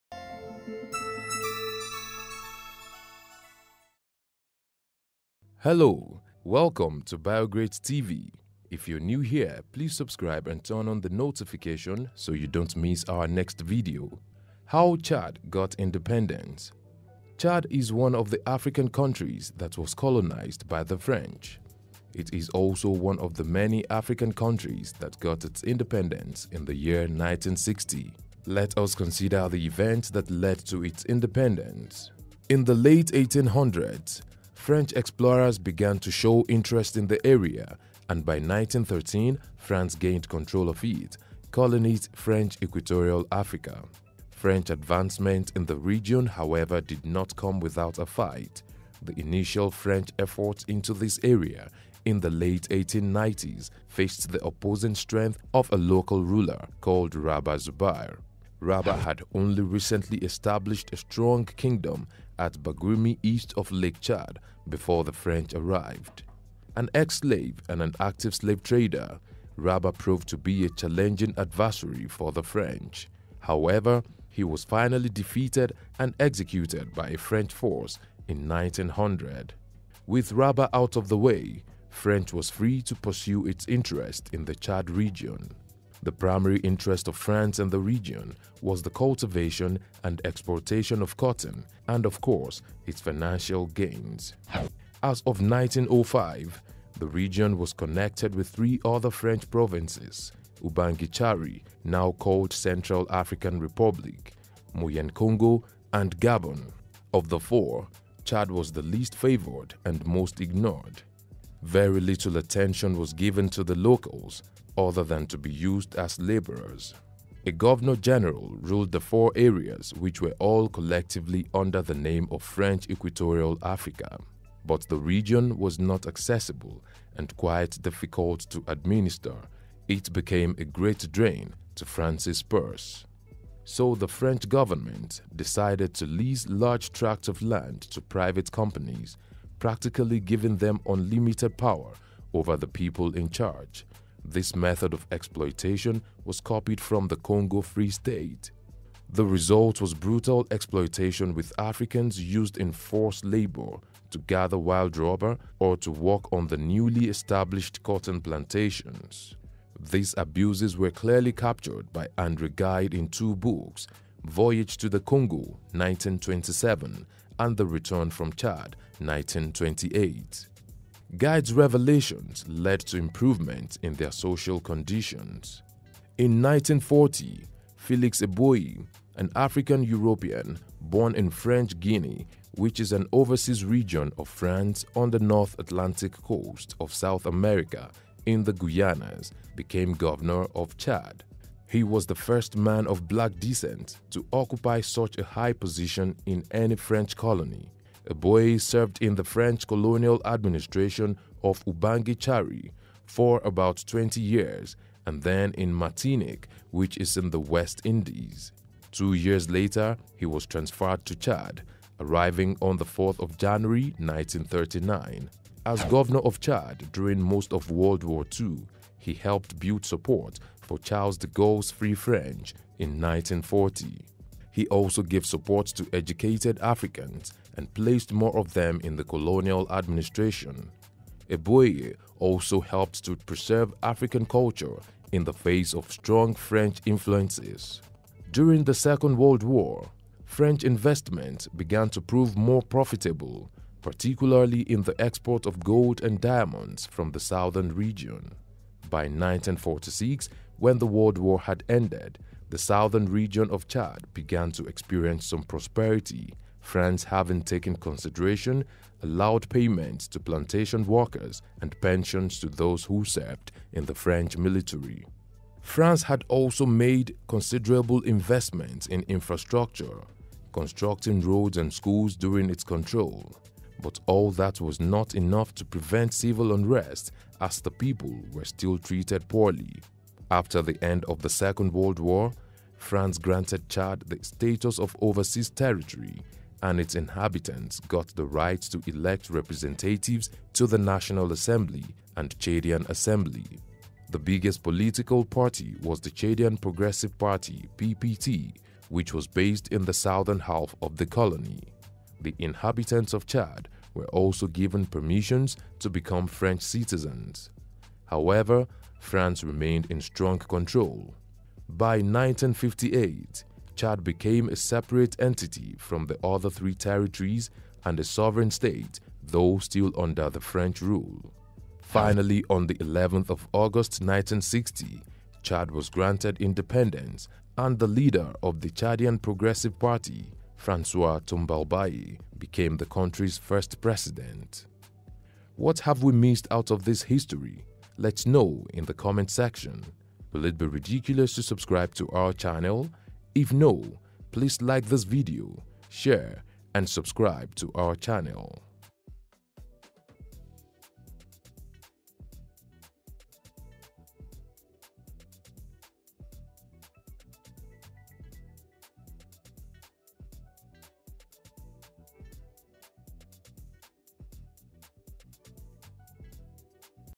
How Chad got independence
Chad is one of the African countries that was colonized by the French. It is also one of the many African countries that got its independence in the year 1960. In the late 1800s, French explorers began to show interest in the area and by 1913 France gained control of it, calling it French Equatorial Africa.French advancement in the region however did not come without a fight. The initial French efforts into this area in the late 1890s faced the opposing strength of a local ruler called Rabah Zubayr.Rabah had only recently established a strong kingdom at Baguirmi east of Lake Chad, before the French arrived. How did Chad get independence? Watch!